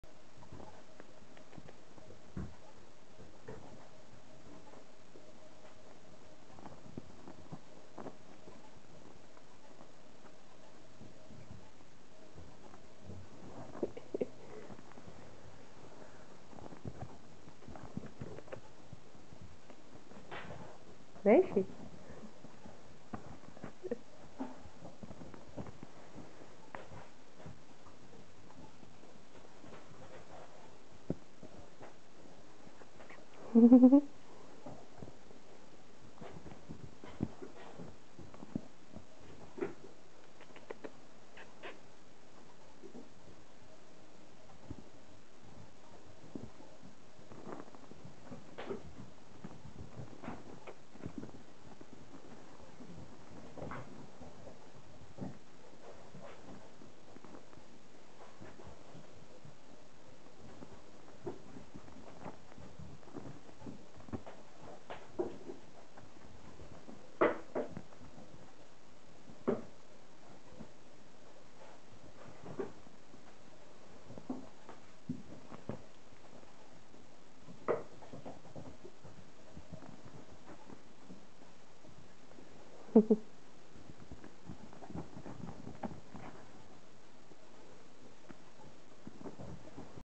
Nijs & Nelis playing together
Sacred Birman kittens 7,5 weeks old just having fun during their "crazy hour" in the morning.